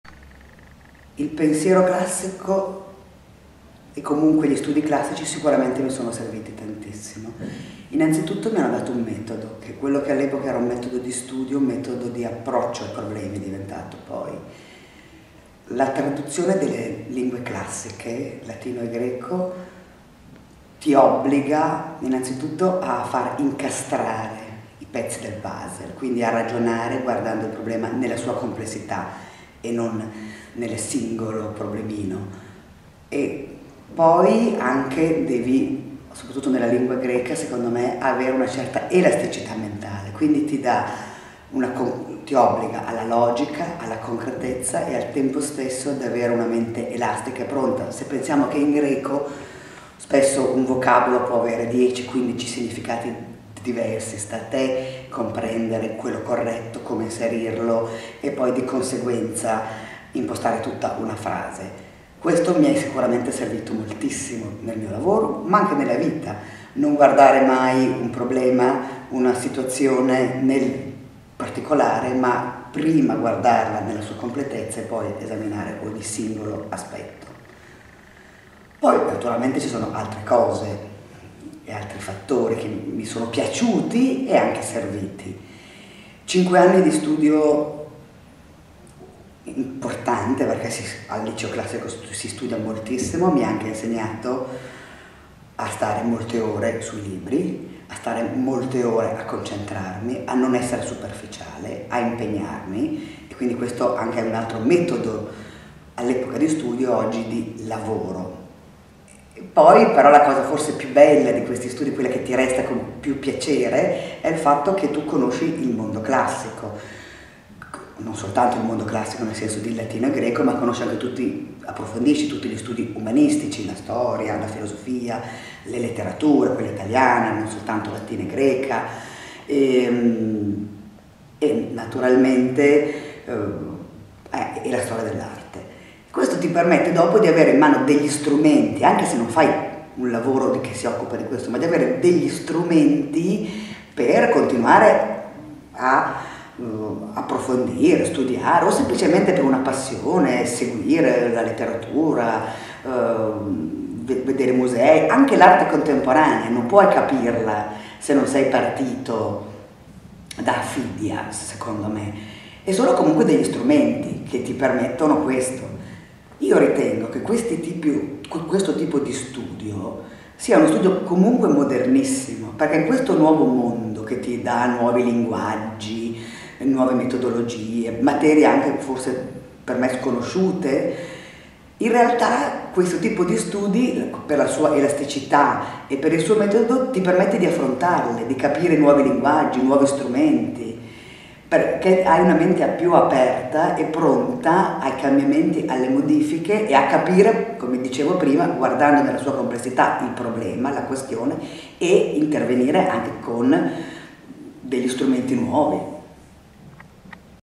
Istituto Marcelline Tommaseo Milano | Scegliere il Liceo classico | Alessandra De Vita Galeone #2
Risposta 2: In che modo le sono serviti la cultura e il pensiero classici nella sua carriera lavorativa? E nella sua
vita?
Risponde Alessandra De Vita Galeone, testimonial ed ex alunna del Liceo Classico a Milano. Alessandra De Vita Galeone, laureata in Giurisprudenza, è avvocato.